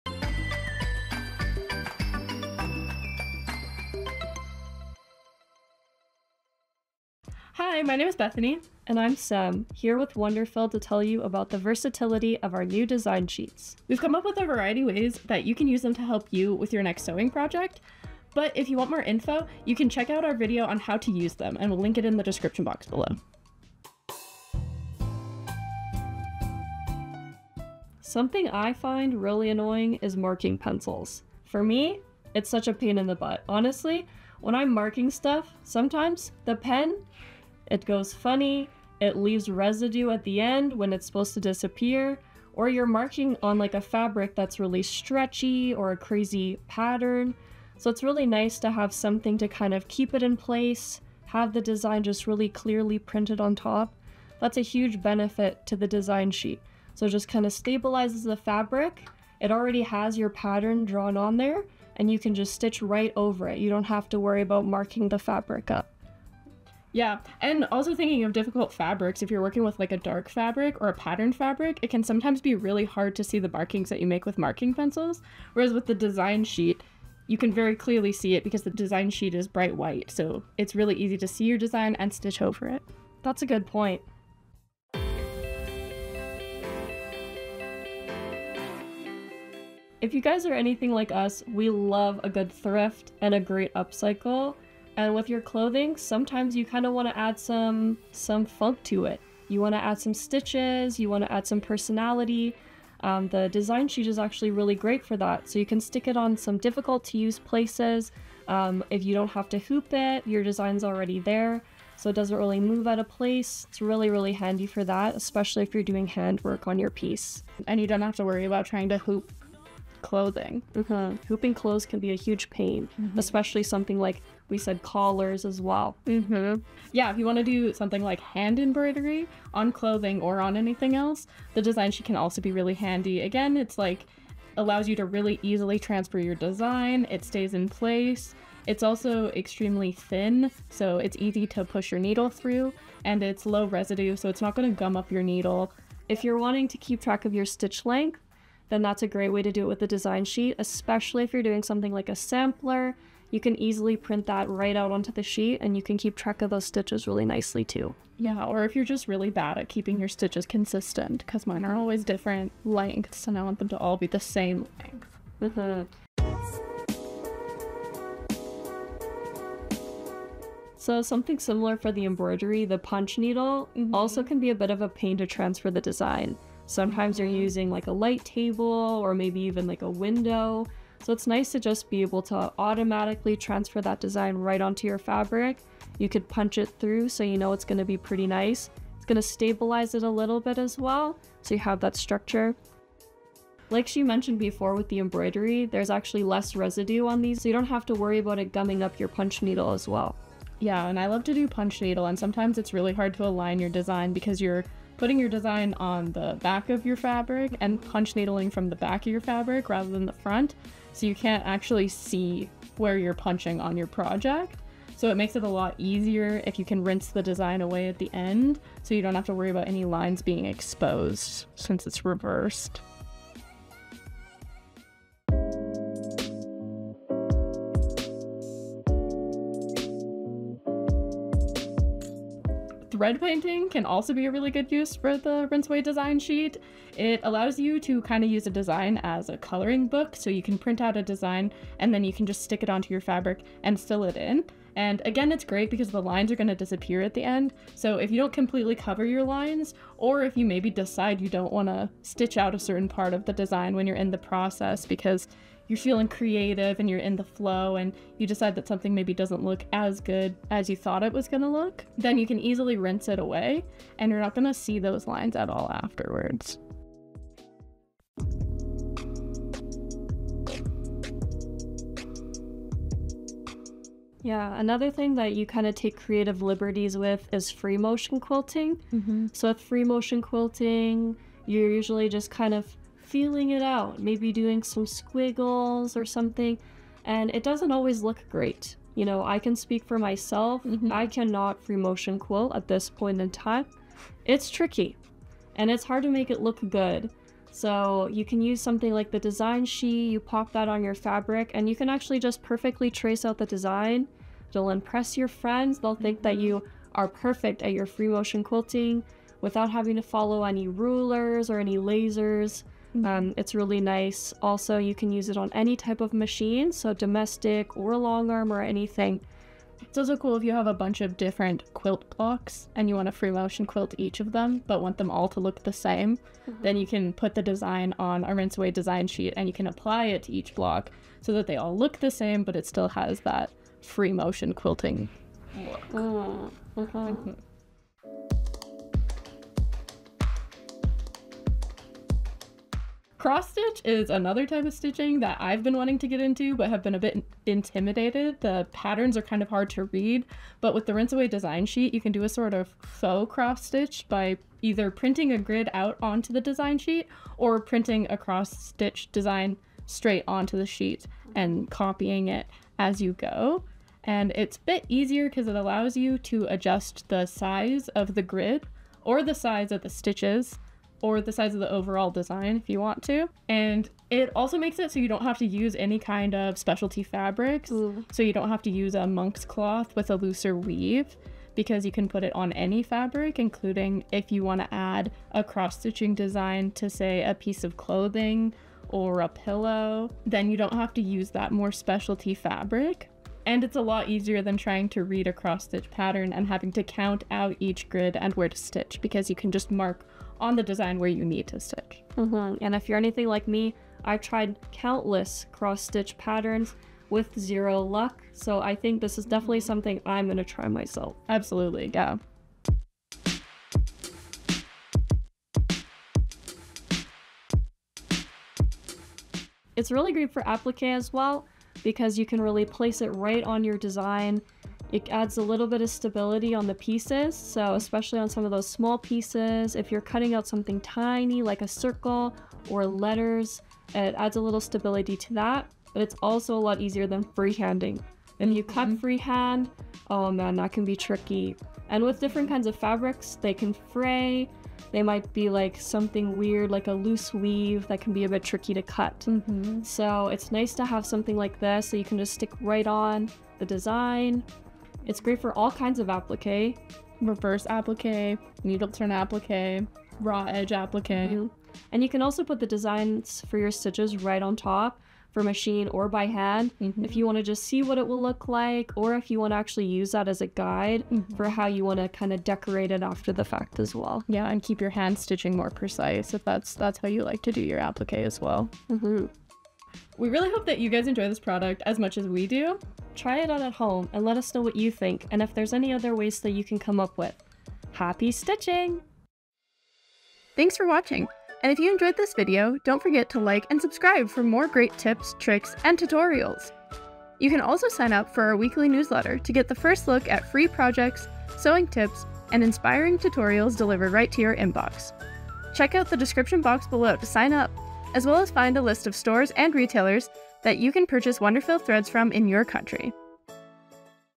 9 Ways To Use The WonderFil Design Sheet: Hand Embroidery, Applique, Thread Painting, & More
The WonderFil Rinse Away Design Sheet is an innovative, new product that offers a seamless way for creatives to transfer their designs, with precision and ease. Opening up new possibilities for hand embroidery, thread painting, clothing design, quilting, and more. Print a design straight from your computer, trace a design onto the sheet by hand, or free hand a design of your own! Then position in place for easy stitching 😱 Let's cover 9 creative ways you can use this fun new product for your upcoming projects ❤️

✨What do we cover in this video? ✨
00:00 Intro
00:58 Marking Pencils
01:12 Dark Patterns
01:27 Clothing
01:17 Hand Embroidery
02:47 Punch Needle
03:51 Thread Painting
04:29 Free Motion Quilting
05:41 Cross Stitch
06:55 Appliqué

Want to have sewing tips and tutorials delivered directly to your inbox? Sign up for our educational newsletter, delivered weekly, for unique content and project ideas: